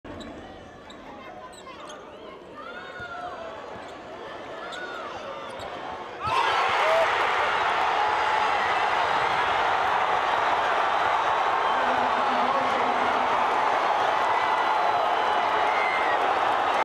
USM's Neil Watson Hits Dagger Three-Pointer Against Marshall
Senior point guard Neil Watson connects with a dagger three-pointer with 2.0 seconds remaining, propelling Southern Miss past Marshall 60-57 on Friday night (2/7/2014).